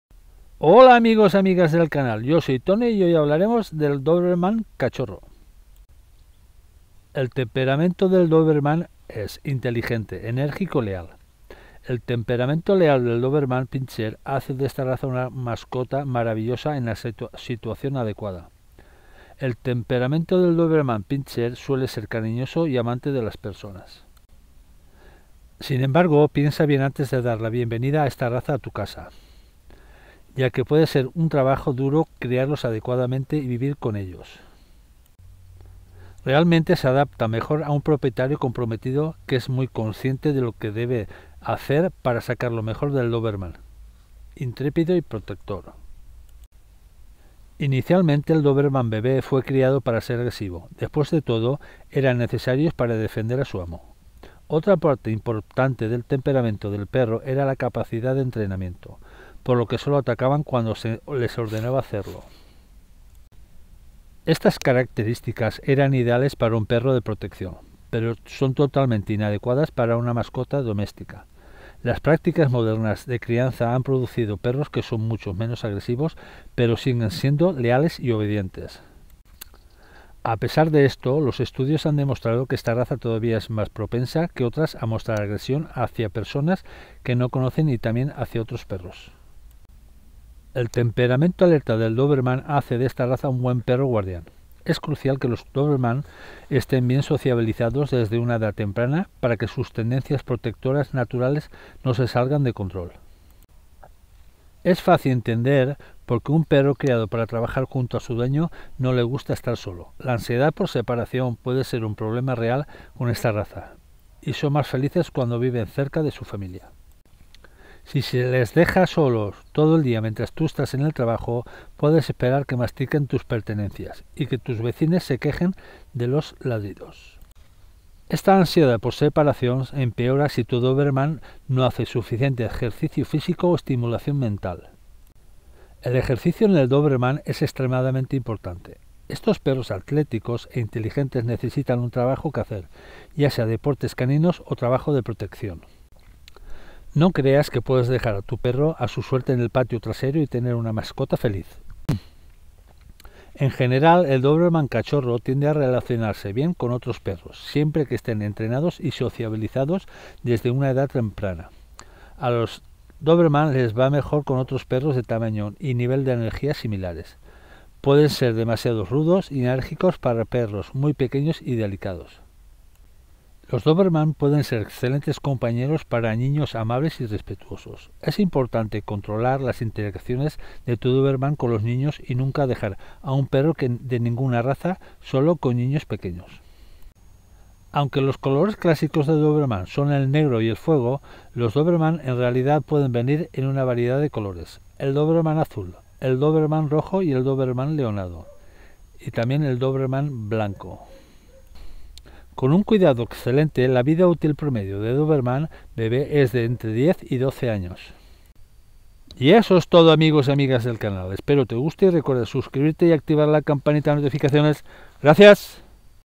✅DOBERMAN Cachorro ✅ bebe,cria de doberman,marron,cafe,orejas cortadas,europeo,como educarlo
hola amigos y amigas del canal yo soy toni y hoy hablaremos del

doberman cachorro

El temperamento del dóberman es: inteligente, enérgico y leal

El temperamento leal del Doberman Pinscher hace de esta raza una mascota

maravillosa en la situación adecuada. , el temperamento del Doberman Pinscher

suele ser cariñoso y amante de las personas.

Sin embargo, piensa bien antes de dar la bienvenida a esta raza a tu

casa, ya que puede ser un trabajo duro criarlos adecuadamente y vivir con

ellos.

Realmente se adaptan mejor a un propietario comprometido que es muy

consciente de lo que se debe hacer para sacar lo mejor del Doberman.

Intrépido y Protector

Inicialmente, el Doberman bebe fue criado para ser agresivo ; después de todo,

eran necesarios para defender a su amo. Otra parte importante del temperamento

del perro era la capacidad de entrenamiento, por lo que solo atacaban cuando se

les ordenaba hacerlo.

Estas características eran ideales para un perro de protección, pero son

totalmente inadecuadas para una mascota doméstica. Las prácticas modernas de

crianza han producido perros que son mucho menos agresivos pero siguen siendo

leales y obedientes.

A pesar de esto, los estudios han demostrado que esta raza todavía es más

propensa que otras a mostrar agresión hacia personas que no conocen y también

hacia otros perros.

El temperamento alerta del Doberman hace de esta raza un buen perro

guardián. Es crucial que los Doberman estén bien socializados desde

una edad temprana para que sus tendencias protectoras naturales no se salgan de

control.

Es fácil entender por qué a un perro criado para trabajar junto a su dueño no

le gusta estar solo. La ansiedad por separación puede ser un problema real con

esta raza, y son más felices cuando viven cerca de su familia.

Si se les deja solos todo el día mientras tu estás en el trabajo, puedes

esperar que mastiquen tus pertenencias y que tus vecinos se quejen de los

ladridos.

Esta ansiedad por separación empeora si tu Doberman no hace suficiente ejercicio

físico o estimulación mental .

El ejercicio en el Doberman es

extremadamente importante .

Estos perros atléticos e inteligentes necesitan un trabajo que hacer, ya sea

deportes caninos o trabajo de protección.

No creas que puedes dejar a tu perro

a su suerte en el patio trasero y tener una mascota feliz.

En general, el Doberman cachorro tiende a relacionarse bien con otros perros,

siempre que estén entrenados y socializados desde una edad temprana. A los

Doberman les va mejor con otros perros de tamaño y nivel de energía similares.

Pueden ser demasiado rudos y enérgicos para perros muy pequeños y delicados.

Los dóberman pueden ser excelentes compañeros para niños amables y respetuosos.

Es importante controlar las interacciones de tu dóberman con los niños y nunca

dejar a un perro de ninguna raza solo con niños pequeños.

Aunque los colores clásicos de Doberman son el negro y el fuego, los

Doberman en realidad pueden venir en una variedad de colores.

 el dóberman azul, el dóberman  rojo y el dóberman

leonado y tambien el Doberman blanco

Con un cuidado excelente, la vida útil promedio de Doberman bebe es de

entre 10 y 12 años.

y activar la campanita de notificaciones gracias